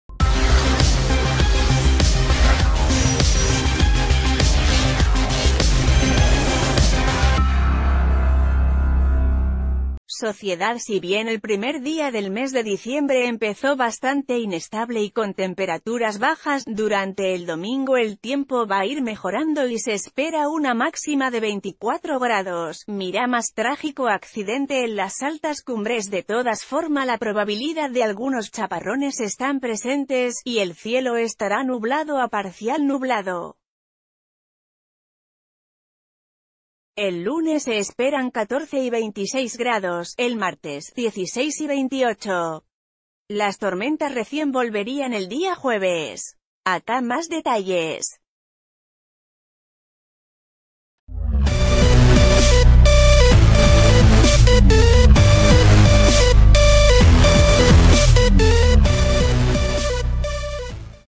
¿Cómo seguirá el tiempo el fin de semana?
Sociedad Si bien el primer día del mes de diciembre empezó bastante inestable y con temperaturas bajas, durante el domingo el tiempo va a ir mejorando y se espera una máxima de 24 grados. +MIRÁ MÁS: Trágico accidente en las Altas CumbresDe todas forma la probabilidad de algunos chaparrones están pre...